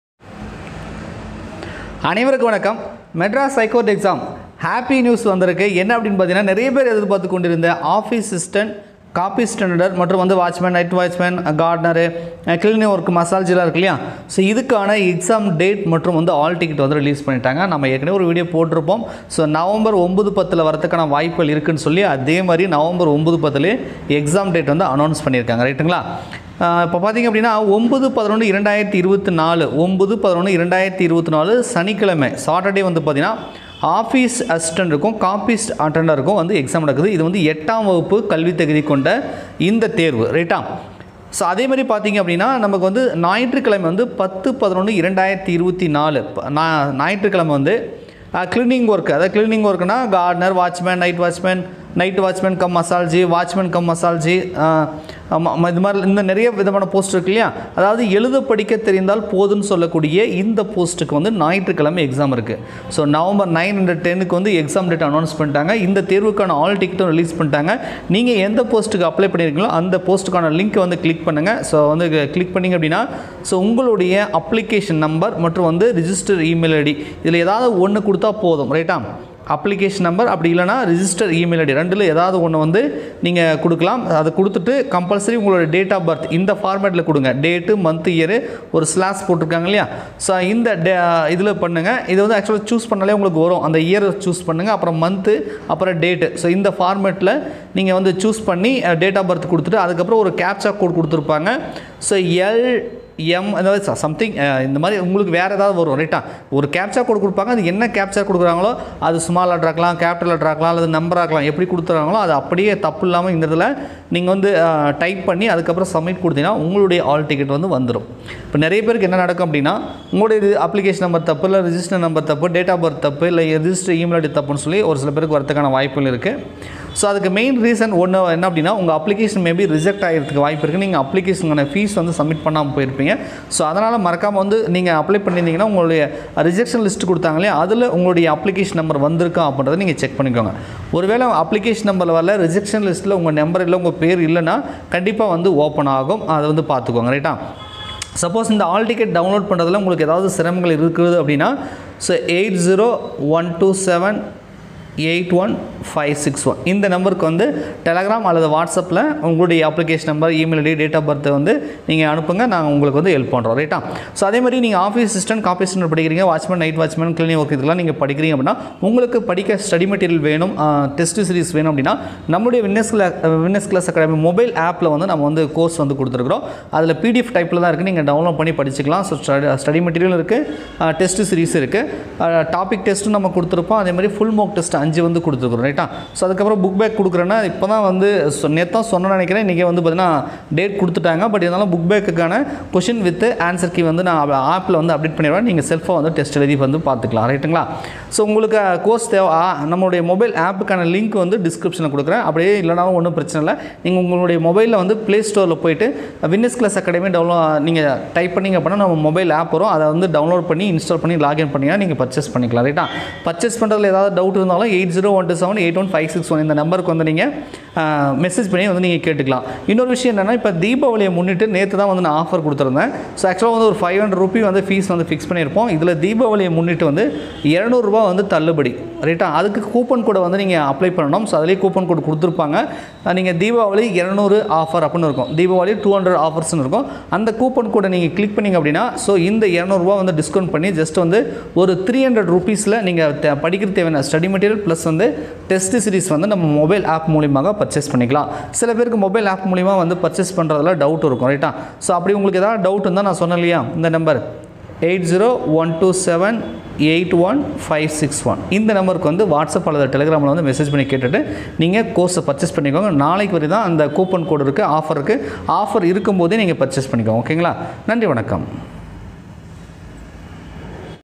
Madras high court hall ticket 2024 out/ exam date/Office assistant/ watchman/ All post
Madras high court office assistant watchman
Hall ticket exam date out
Exam date: November 9,10